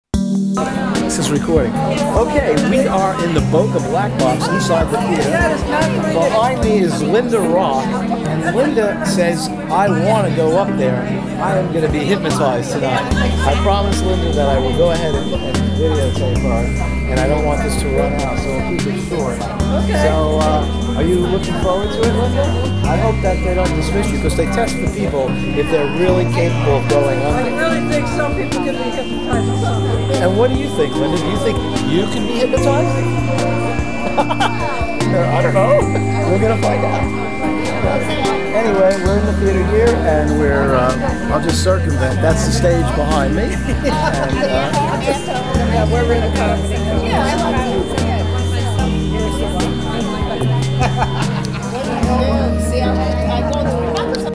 5.) DINNER FOLLOWED BY ADULT HYPNOTIST SHOW AT THE BOCA BLACK BOX MEETUP - FRI., OCT. 21ST, 2016
What a terrific treat of Greek Dining followed by a barrel of belly laughs at the Boca Black Box. The non-stop side-splitting laughter was highlighted by the performance of comic superstar Marion Grodin whose high action, no holes barred humor is reminiscent of Amy Schumer.  As a matter of fact, I spoke with her after her performance to say she reminded me a bit of Amy, and she replied back saying to me she gets that all the time.  She's hysterical & raised the bar for all other comics on stage.  And then Gary Conrad made his hypnotic performance by "casting spells" on some volunteers in the audience.  One who I knew sitting aside me (Hi "L.R") had some video taken on her smart phone that I'd love to see posted here of this "R-Rated Adult" hypnosis that had some Meetup members twisted together like pretzels on stage experiencing "climaxes," while others were running about the theater in their underwear doing sexy lap dances to those sitting in the audience! Oh What A 5-Star Night Out!!!  :)      :)         :)